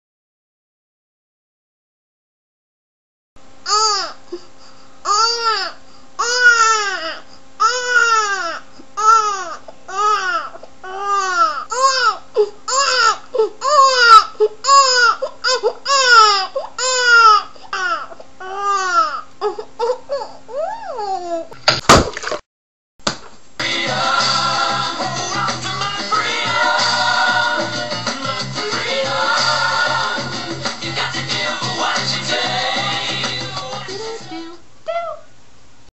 Cry Baby
What do MOST mothers do when their sweet little baby needs something? Hopefully NOT this! :)
WARNING: If you are "squeamish", love babies, or just can't take a joke you MAY be offended by this,.